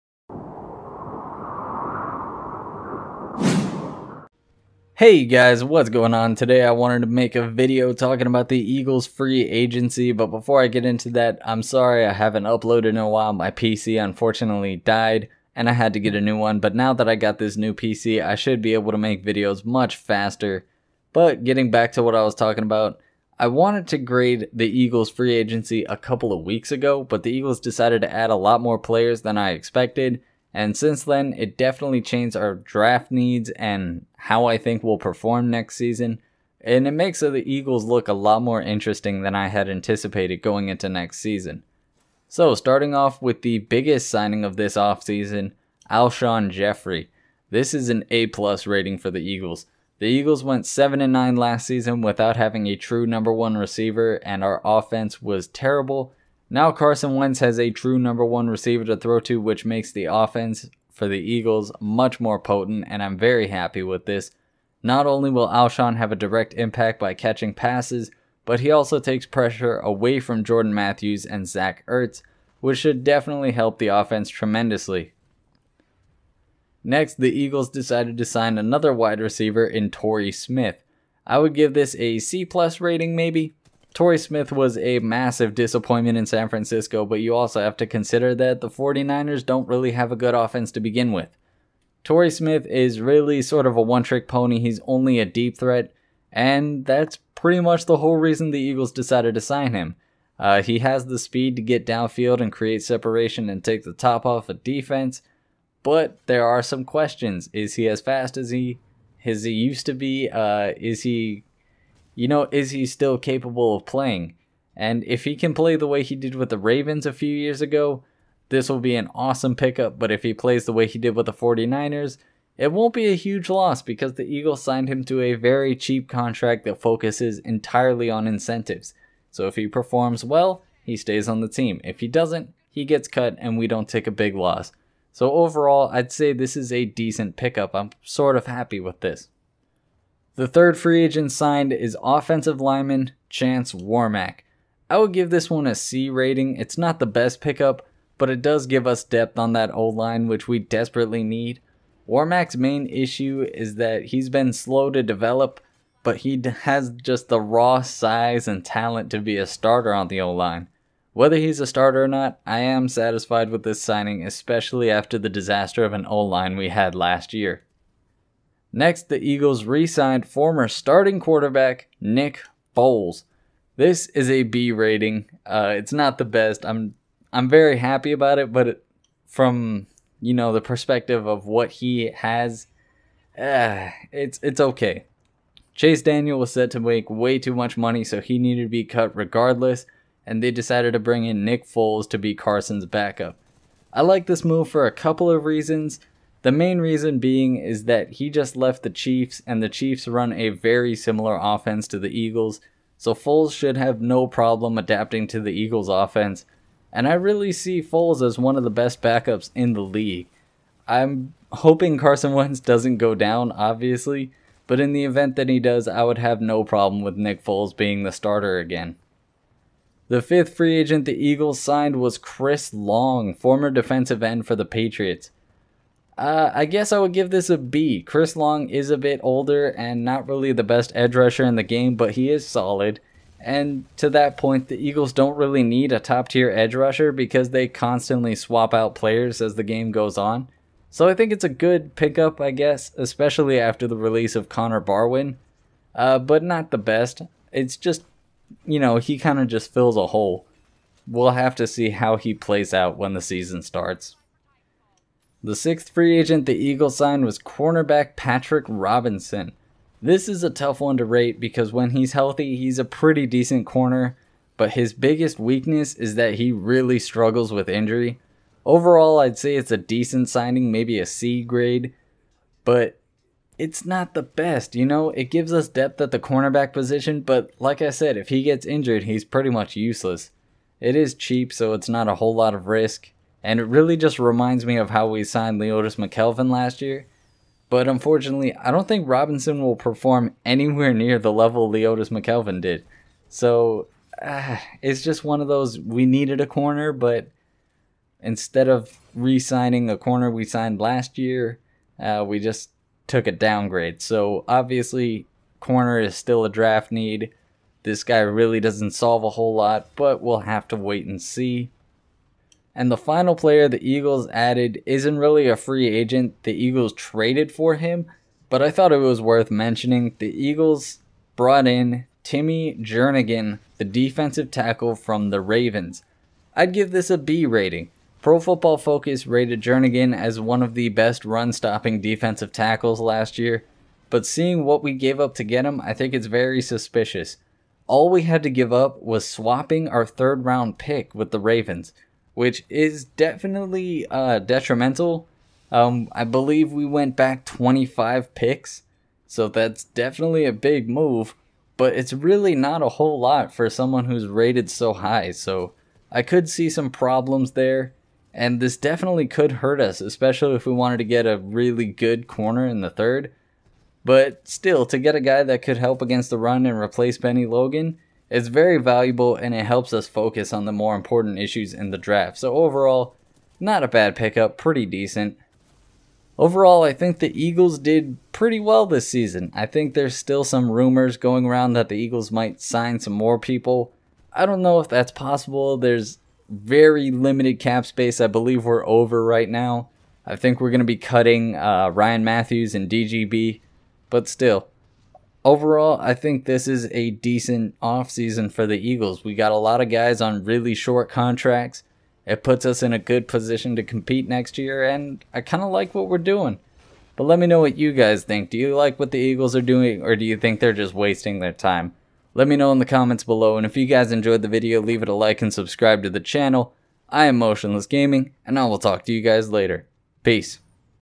Grading Eagles Free Agency 2017
The Eagles made some huge moves this offseason in signing seven new players. Each player is on a short term deal and very cheap contracts. The biggest signing obviously is Alshon Jeffery. Alshon Jeffery signing with the Eagles is a massive impact, obviously we will have to wait and see how he performs with the team to see if he gets a long term deal but so far the signing is great for the Eagles. The other signings for the Eagles include Torrey Smith, Chance Warmack, Nick Foles, Patrick Robinson, Chris Long, and Timmy Jernigan. Timmy Jernigan is another very interesting pickup. The Eagles swaped 3rd round picks with the Ravens in order to pick up Timmry Jernigan. I am suspicious of this move because Jernigan is a very good run stopping defensive tackle and the Ravens didn't really get a whole lot of value for him. People have suspected it was due to a lack of work ethic and inconsistencies with his weight. So we will have to see if he has his mind set on being with the Eagles or if he will just be a guy to take some of the load off of Beau Allen.